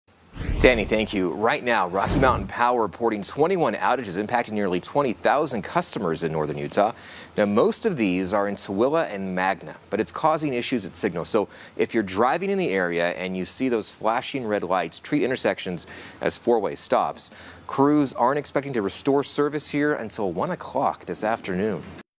Over 19K without power in Magna and Tooele
More than 19,000 customers were without power for several hours in northern Utah early Wednesday morning, mostly in Magna and Tooele County.